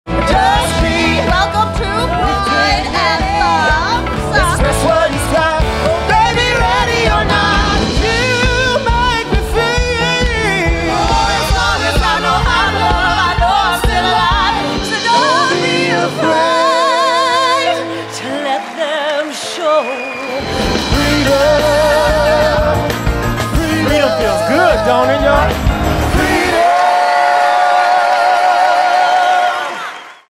Pride Anthems at the Adrienne Arsht Center
Join us as we celebrate Pride Month with a journey through the past 50 years of Pride anthems. From Donna Summer to Queen, George Michael, Erasure, Madonna, Lady Gaga and many others, these essential songs take us from disco to the present day. The story of the fight for LGBTQ+ equality is linked to this music, which evokes the struggles, heartache and liberation of queer lives then and now.

Pride Anthems creates a vibrant musical experience that celebrates, inspires and commemorates the legacy and power of the Stonewall Riots. Starring Natalie Joy Johnson, Kevin Smith Kirkwood and Jon-Michael Reese, with musical director Brian J. Nash, Pride Anthems is a show for all ages to come together in song and celebration of the LGBTQ+ community and how far it has come since Stonewall!